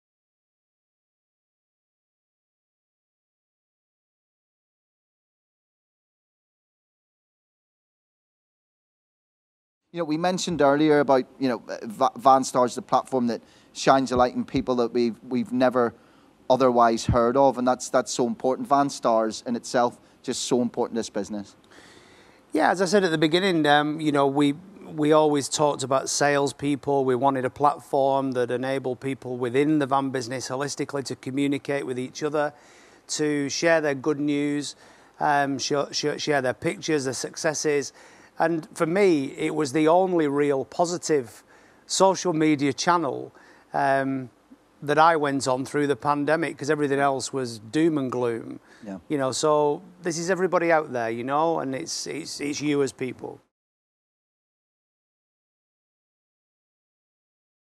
Mercedes-Benz Vans UK's Managing Director, Steve Bridge, talking about the Rippl platform
This is Mercedes-Benz Vans UK's Managing Director in 2020 discussing the importance of their Rippl Incentives, Recognition and Reward platform, Van Stars.